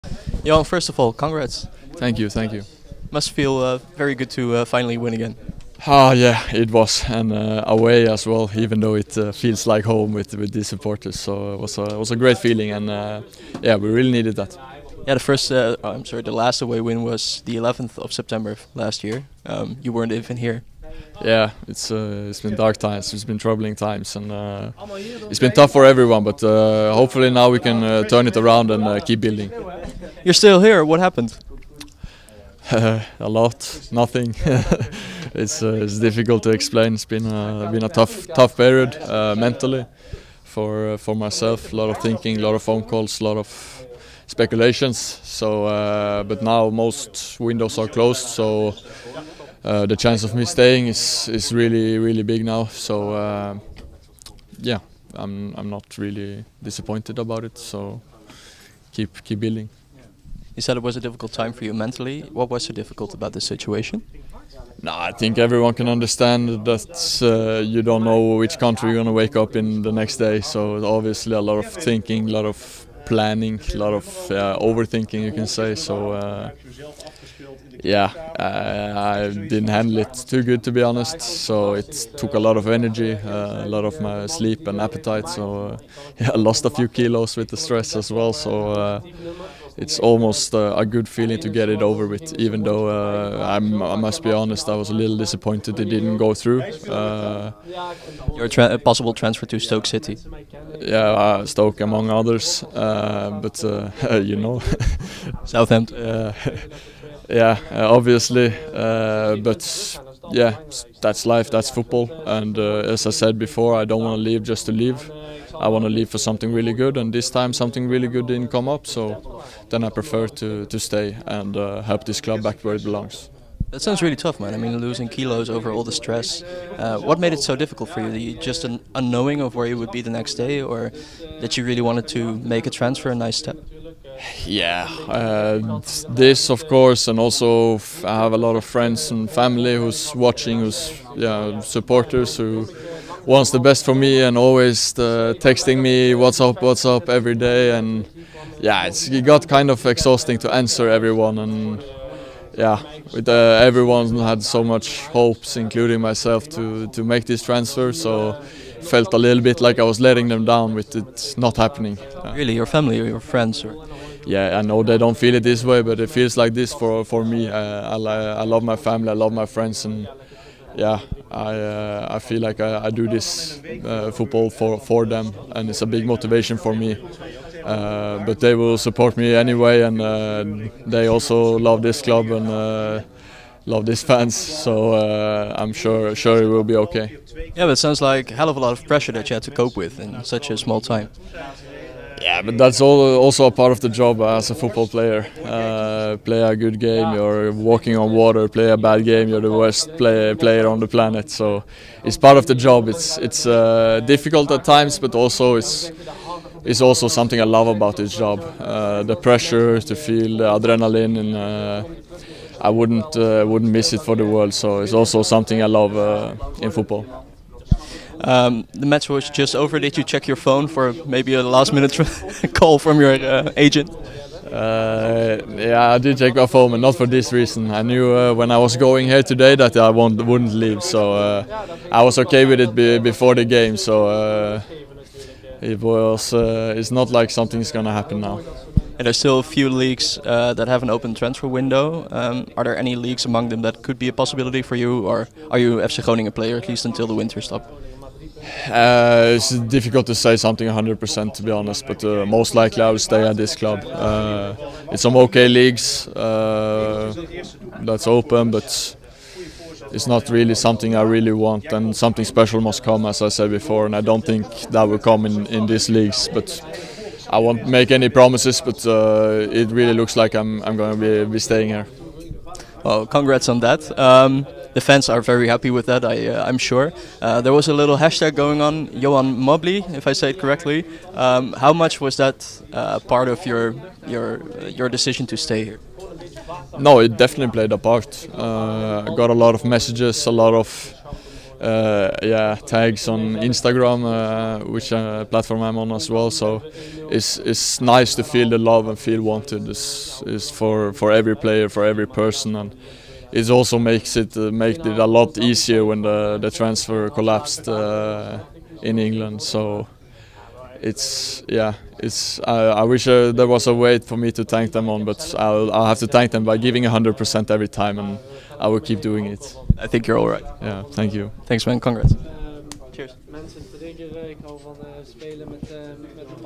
Hove over mislopen transfer!
Johan Hove blijft bij FC Groningen, vertelde hij na TOP Oss - FC Groningen (0-2)